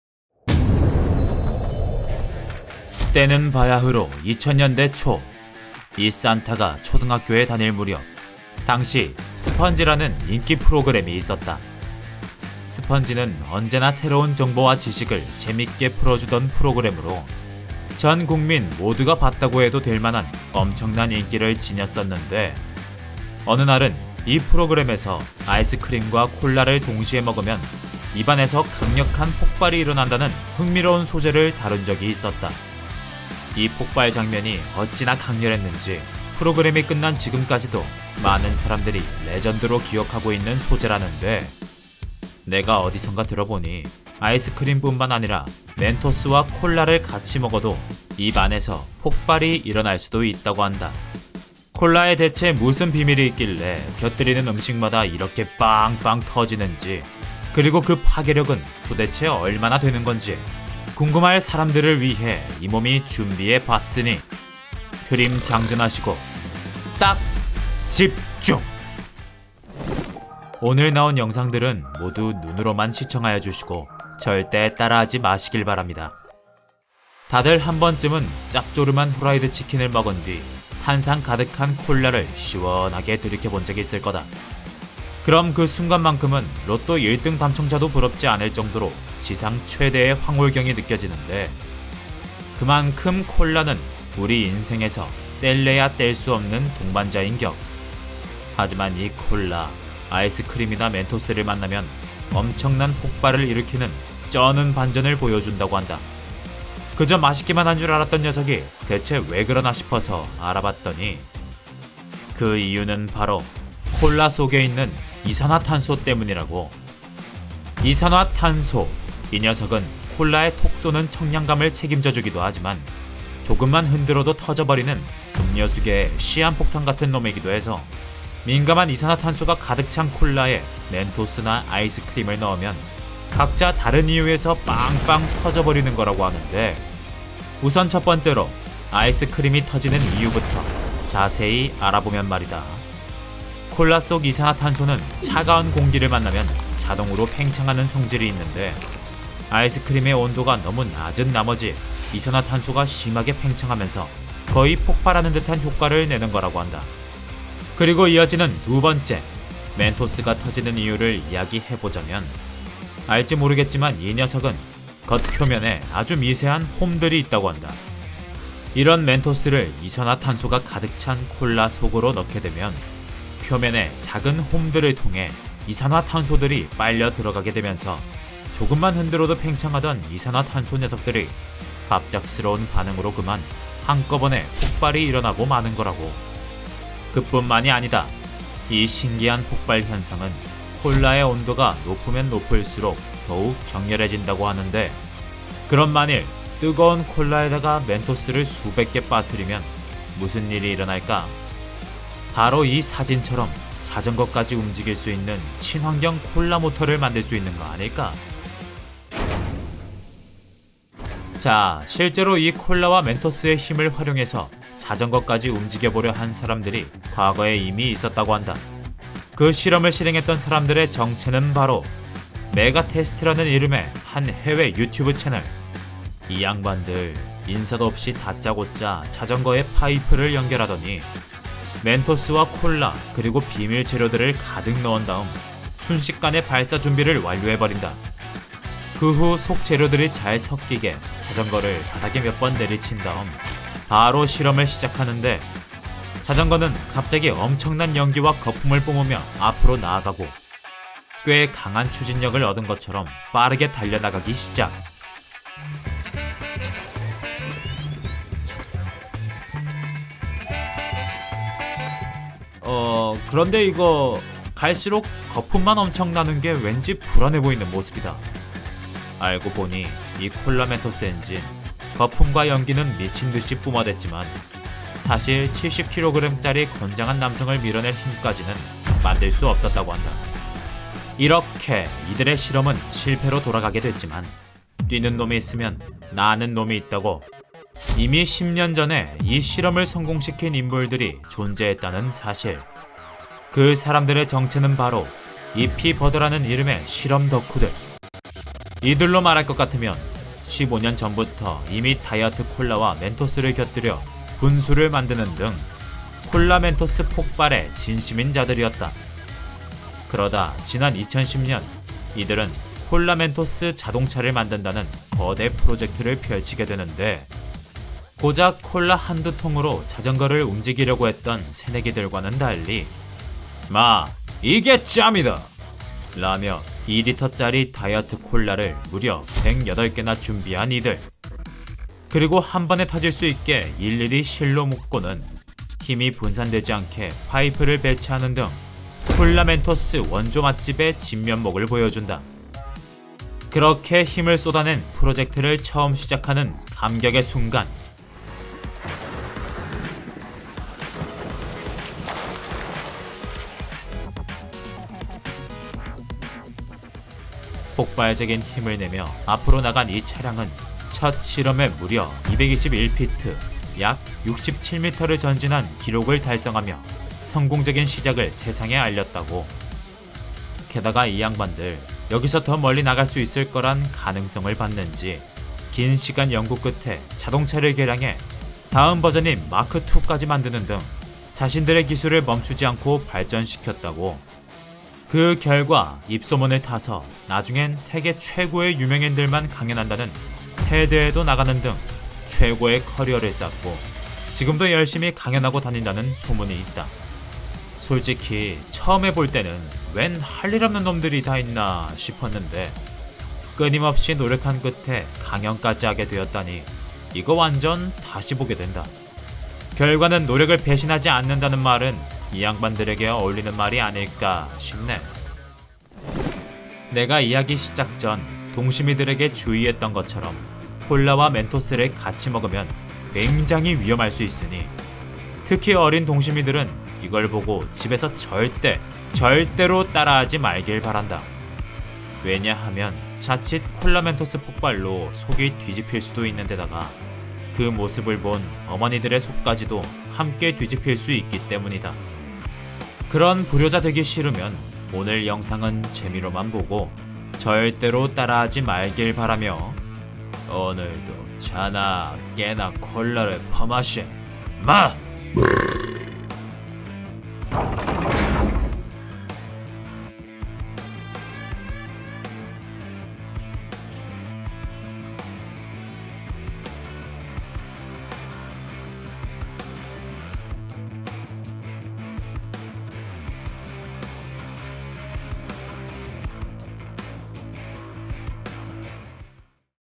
멘토스와 콜라로 자전거 모터를 만들어봤습니다
✒한번 해보실래요?

✒아직도 구매안했으면 당장 구매해야 합니다

VIP 구독자만 볼 수 있는 레전드 비공개 영상 🎅

🥤(콜라) : 터진다 !!!

내 인생 최고의 동반자인
콜라가 폭발할 수 있다고???
이게 무슨 소린지 궁금하다면

딱! 집중

싼타 X 남현